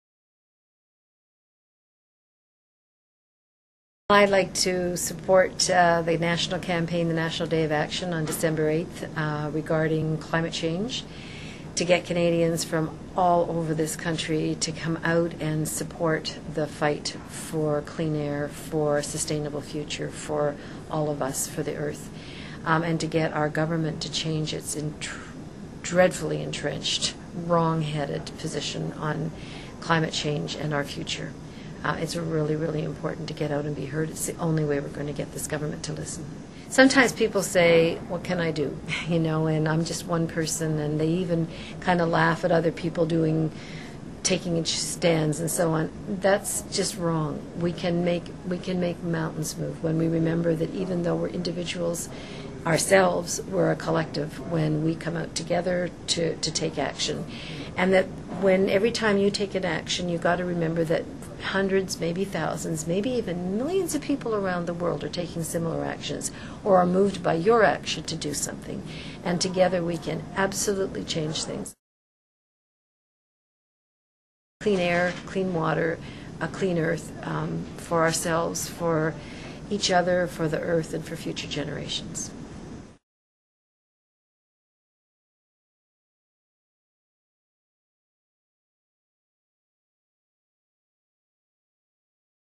Maude Barlow Speaks Out For December 8 Protests
Maude Barlow, National Chairperson of the Council of Canadians, encourages everyone to take to the streets on December 8 to protest the Canadian government's approach to climate change.